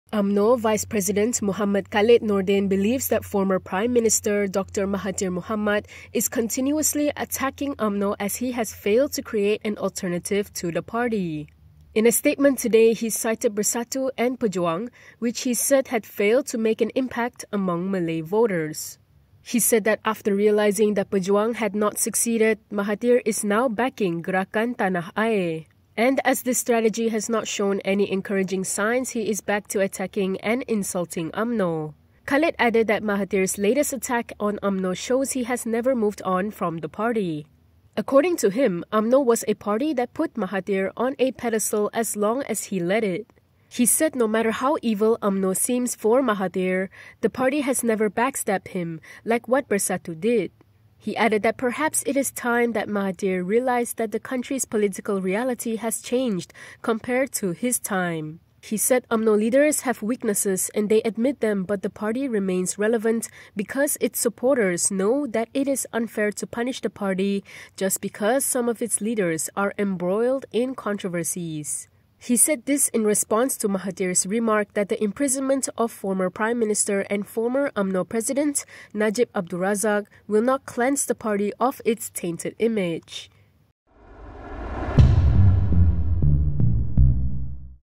Dr M attacking Umno as he has failed to provide an alternative, says Umno veep
Former prime minister Dr Mahathir Mohamad is continuously attacking Umno to cover up his failure to create an alternative to the party, said an Umno leader today.

Party vice-president Mohamed Khaled Nordin cited Bersatu and Pejuang as examples of Mahathir's ventures post-Umno, which he said failed to make an impact among Malay voters.